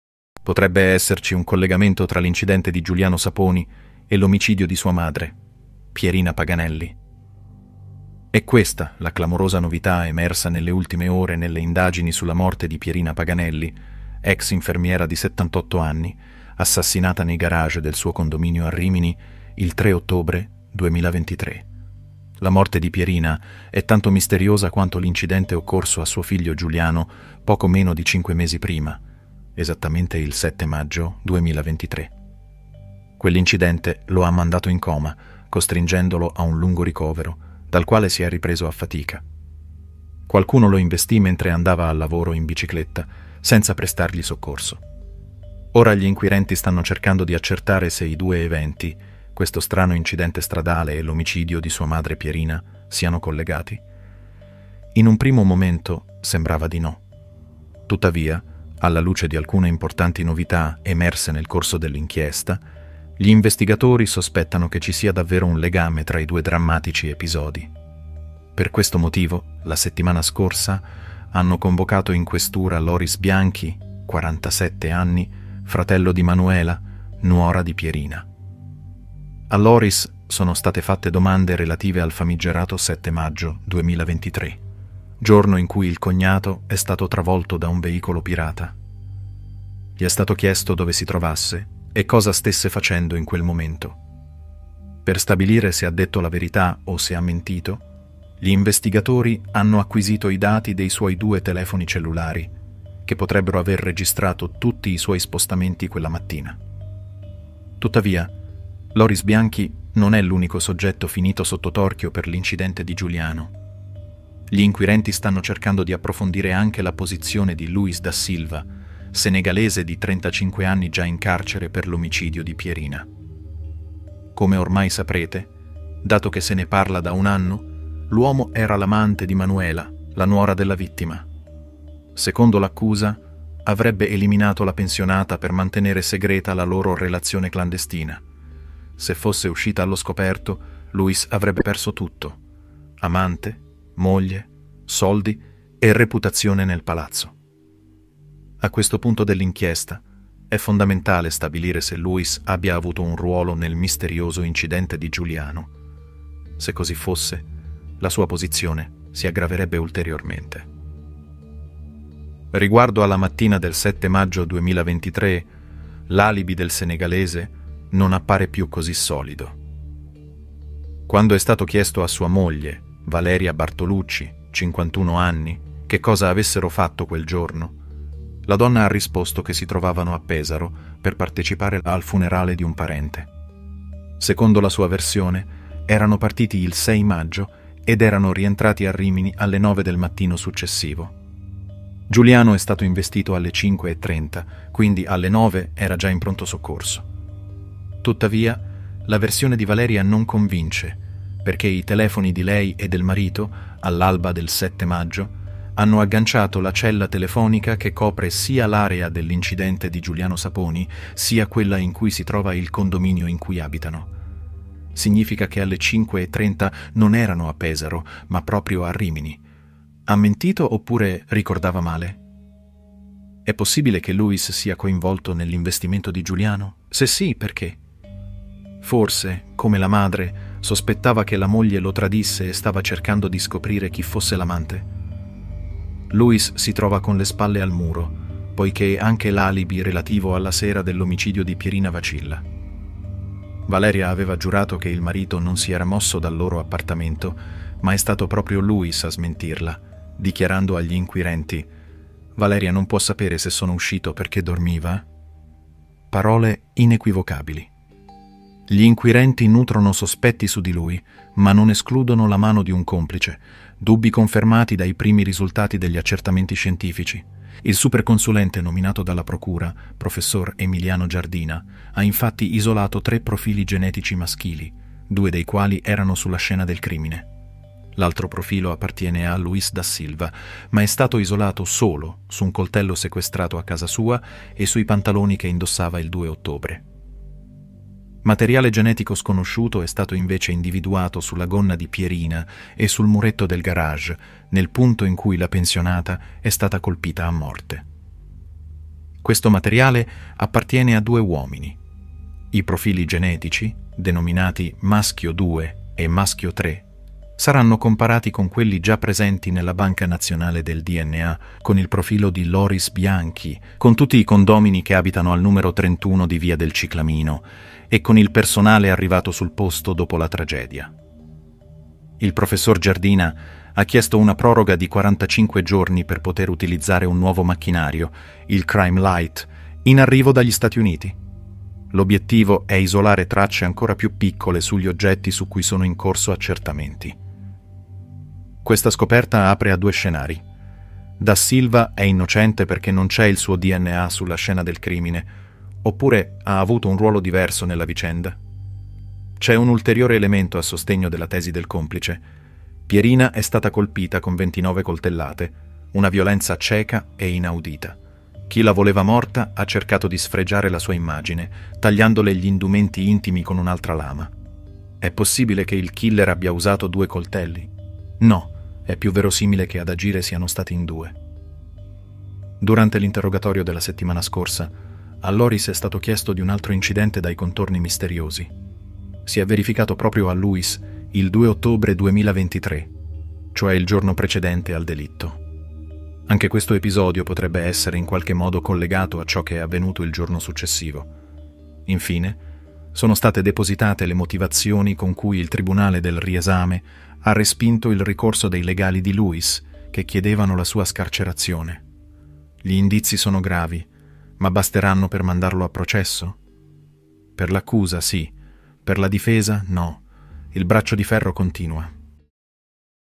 Crescono i sospetti - Pierina Paganelli
Clamorosa novità nel giallo della morte di Pierina Paganelli.